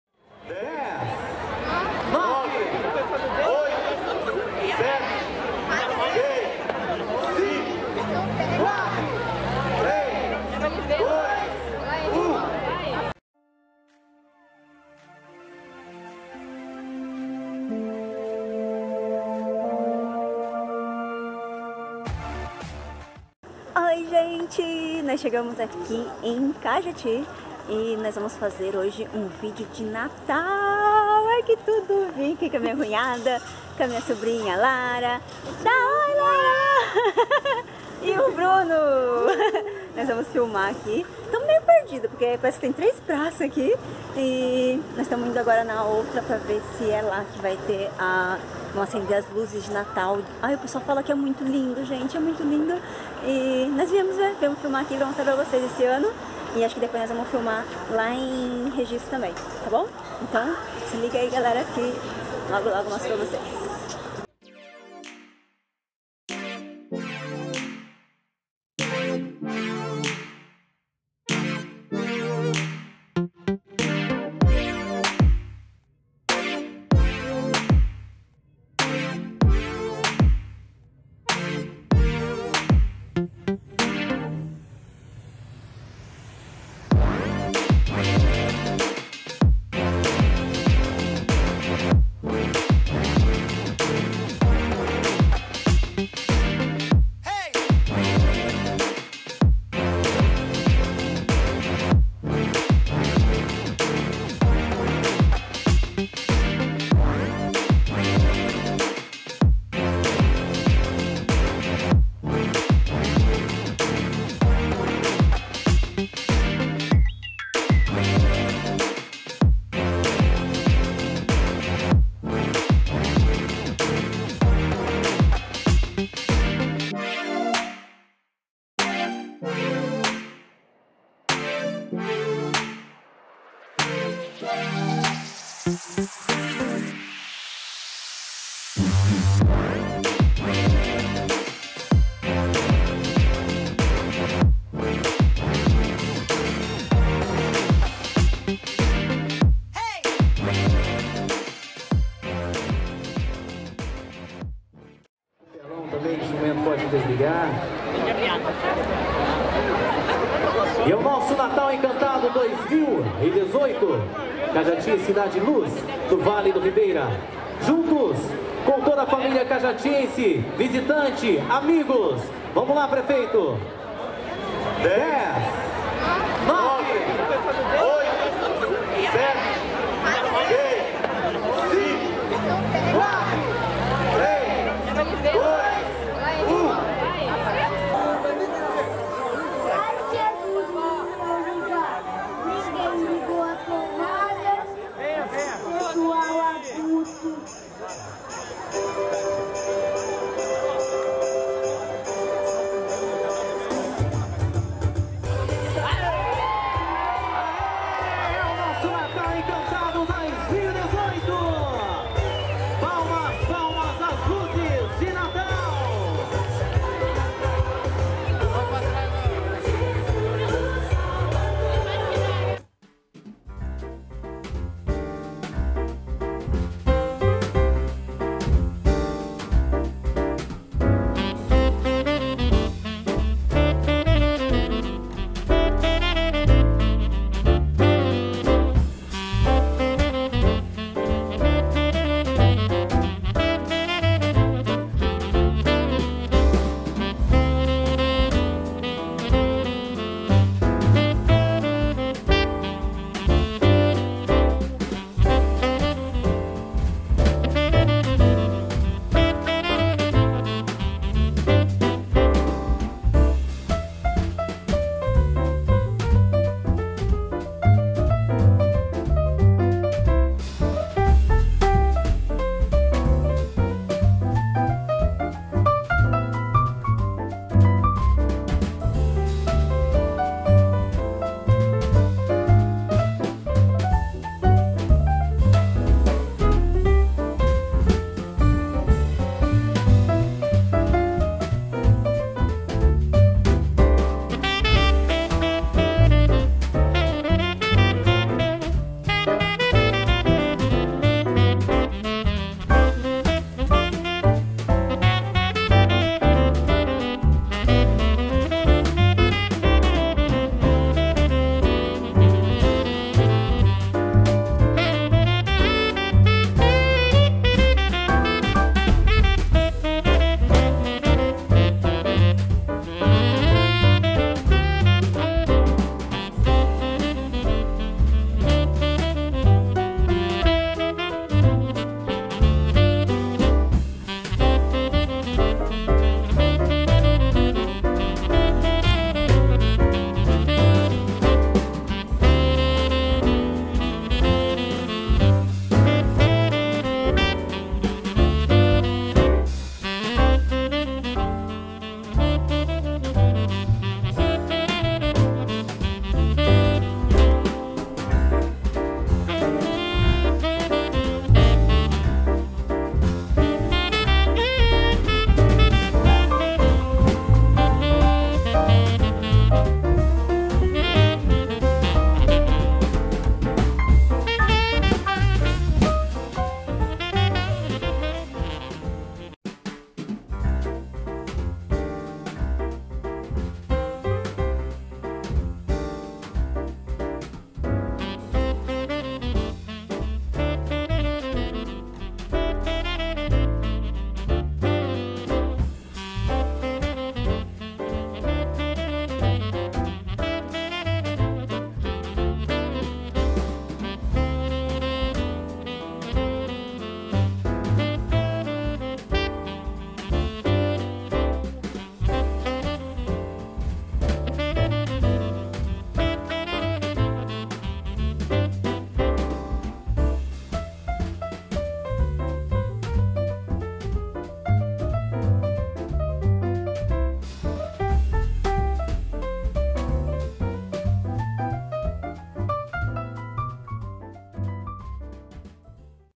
INAUGURAÇÃO LUZES DE NATAL CAJATI 2018
Então é NATAL !!! Época linda do Ano e venho mostrar para vocês a inauguração das Luzes de Natal em Cajati interior de São Paulo :)
Vem que o vídeo está muito lindo e cheio carinho !!!
bjks Rê...